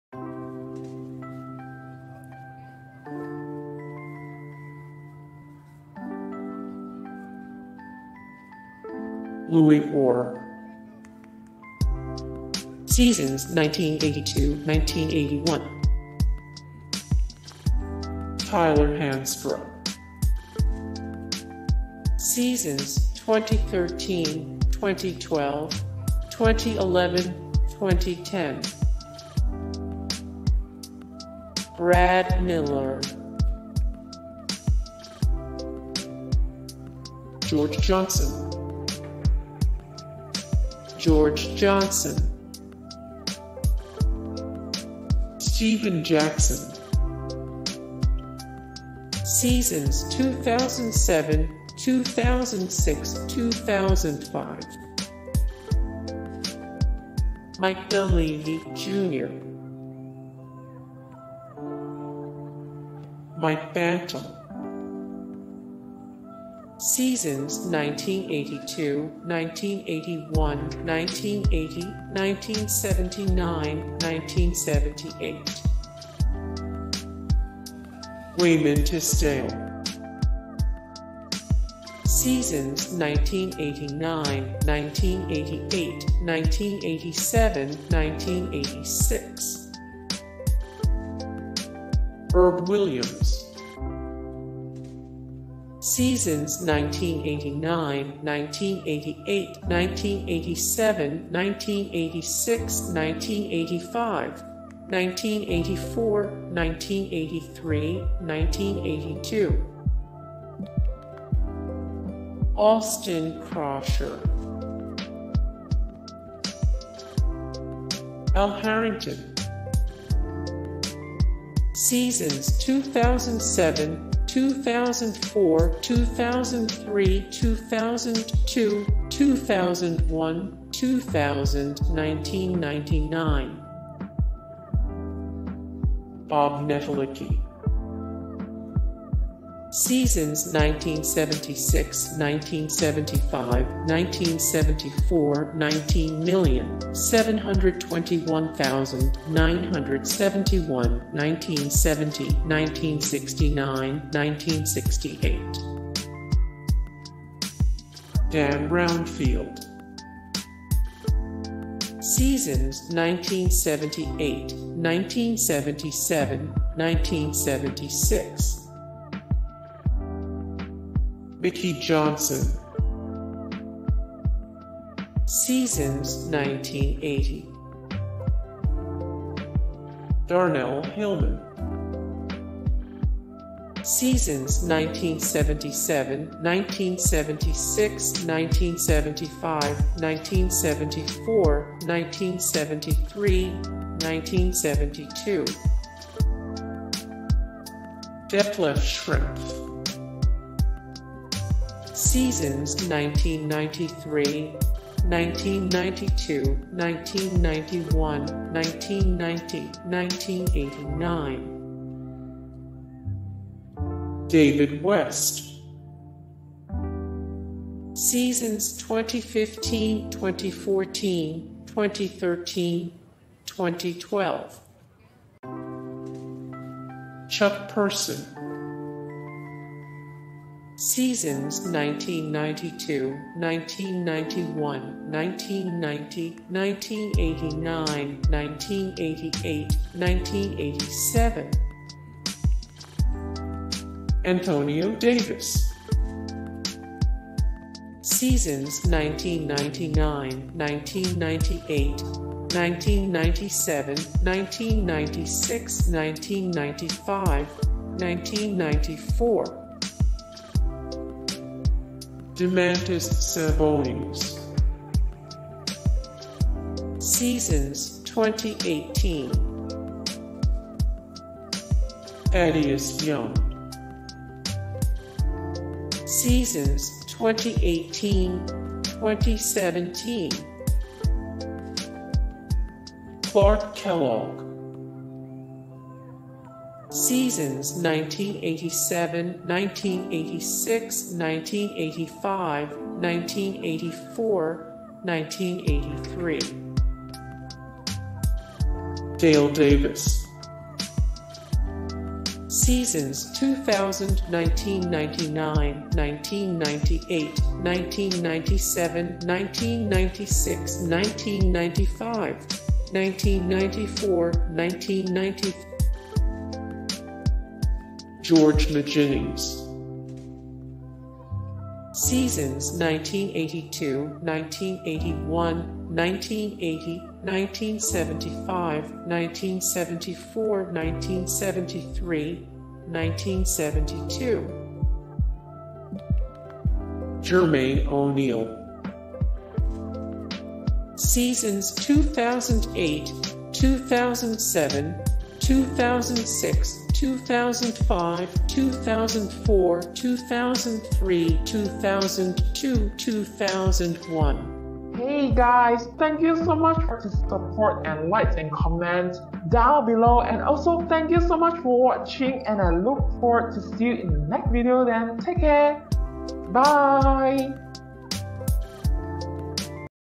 The Best Indiana Pacers Power Forwards Of All Time 💟
Big fan of Indiana basketball? Then check out this list of Indiana Pacers power forwards with photos and seasons played with the team.It includes almost every power forward from throughout Indiana Pacers history, including current power forwards and past ones. Featuring some of thegreatest NBA power forwards of all time,the best PacersPFs include Antonio Davis, David West, Jermaine O'Neal, and Dale Davis.In the 2017-18 NBA season, the current Indiana Pacers starting PFs and backup PFs are Thaddeus Young, Domantas Sabonis, and T. J. Leaf.Who are the best power forwards in Indiana Pacers history? Here is every player who has ever held the position of Indiana Pacerspower forward. more
0:00 - Intro
0:00:08 - Louis Orr
0:00:18 - Tyler Hansbrough
0:00:32 - Brad Miller
0:00:39 - George Johnson
0:00:48 - Stephen Jackson
0:01:01 - Mike Dunleavy, Jr.
0:01:08 - Mike Bantom
0:01:24 - Wayman Tisdale
0:01:39 - Herb Williams
0:02:00 - Austin Croshere
0:02:07 - Al Harrington
0:02:26 - Bob Netolicky
0:02:49 - Dan Roundfield
0:03:03 - Mickey Johnson
0:03:13 - Darnell Hillman
0:03:32 - Detlef Schrempf
0:03:49 - David West
0:04:03 - Chuck Person
0:04:21 - Antonio Davis
0:04:40 - Domantas Sabonis
0:04:51 - Thaddeus Young
0:05:03 - Clark Kellogg
0:05:20 - Dale Davis
0:05:40 - George McGinnis
0:06:00 - Jermaine O'Neal